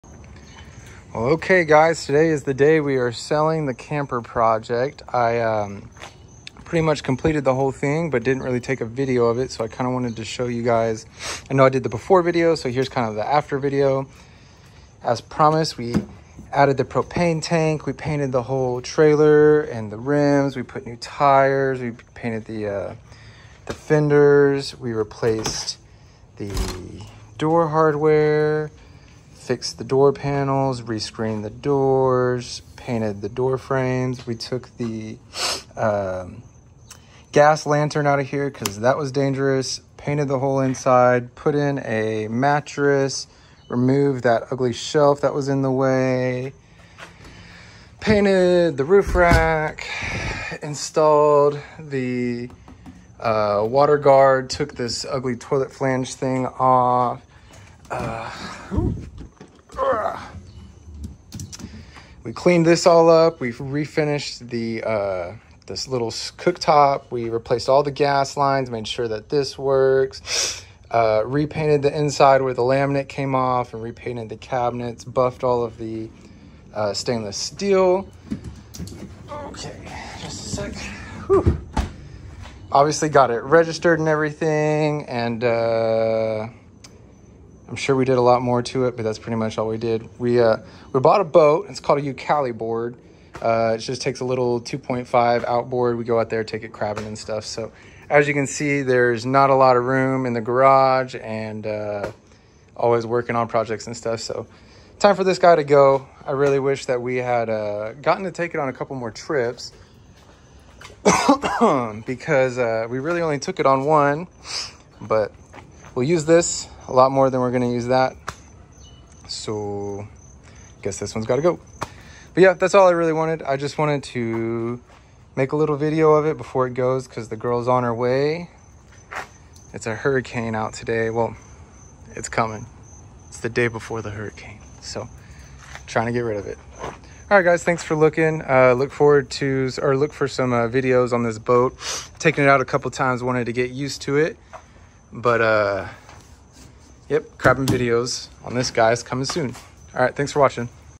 We Sold The 1960 Teardrop Trailer!
Well as much as we wanted to keep the vintage camper trailer around, we decided it was time to sell it. Why? Because we bought a boat and didn't want to try to keep both in the garage. It was a hard decision but ultimately we decided we would use the boat way more often than we would use the camper. This is just a video documenting how it looked when I sold it and to showcase all the things I did to clean it up.